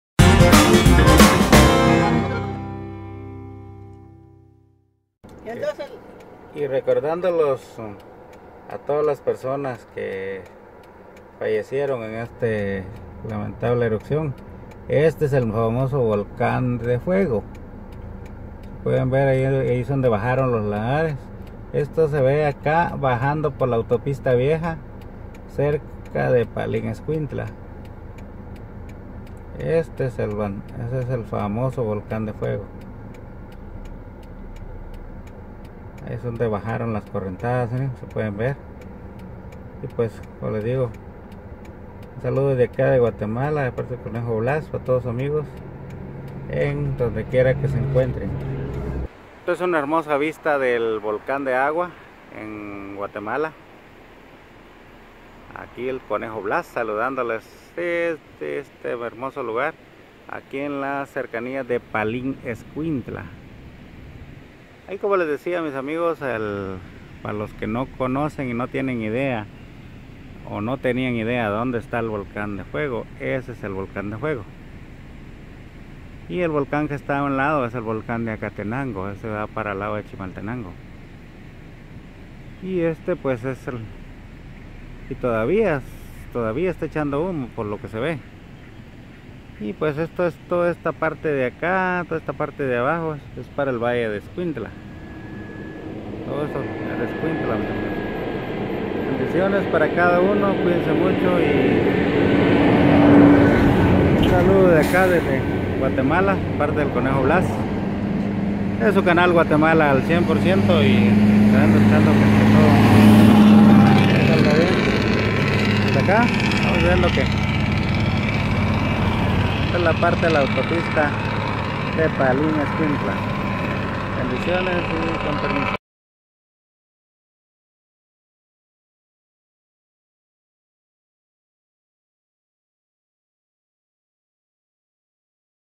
Volcan de Fuego Guatemala
Un hermoso pero peligroso volcán Guatemalteco.
EN MEMORIA DE TODAS LAS VICTIMAS DE LA ERUPCIÓN  DE ESTE COLOSO. Bendiciones para todos.